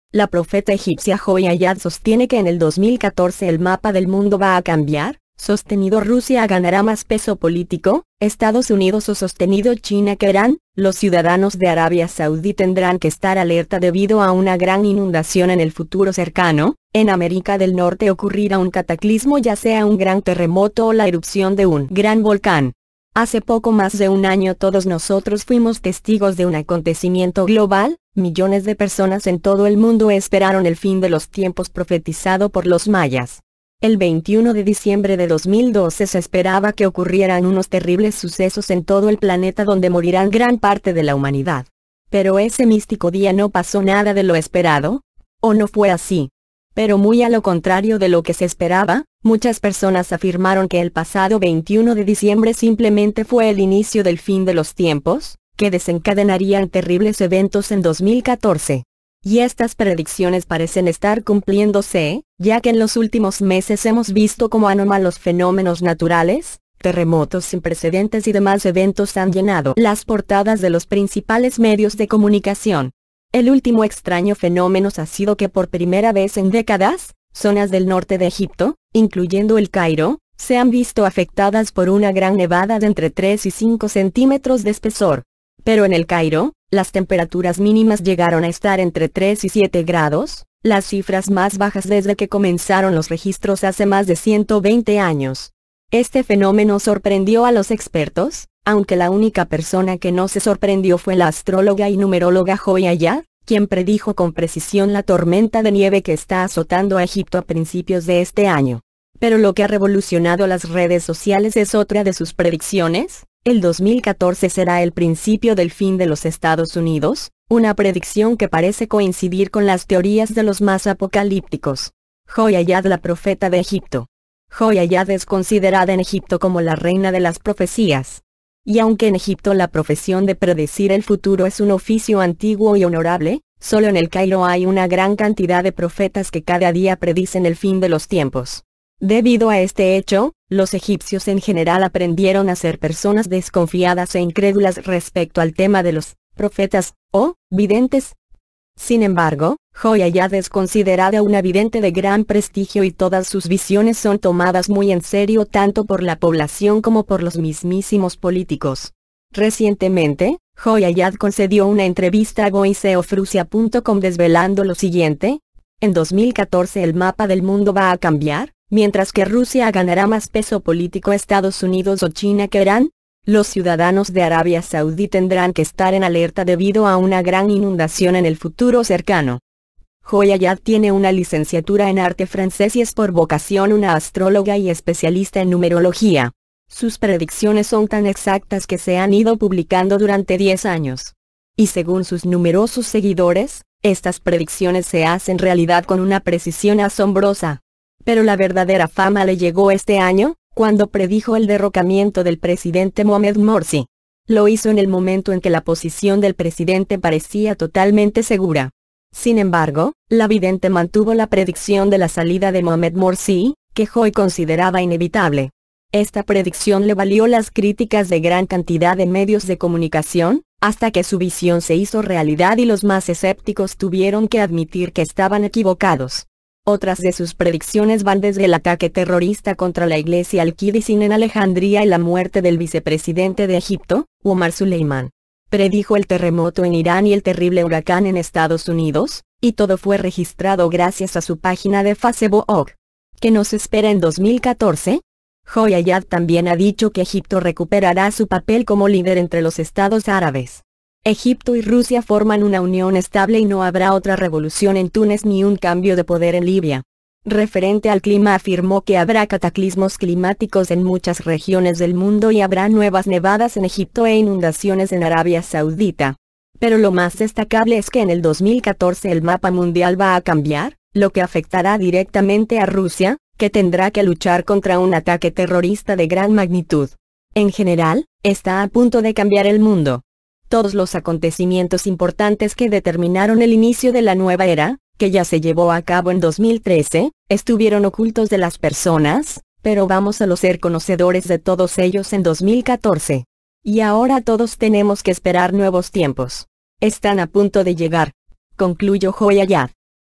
Profeta egipcia asegura que el 2014 será el principio del fin de Estados Unidos
oosveluzo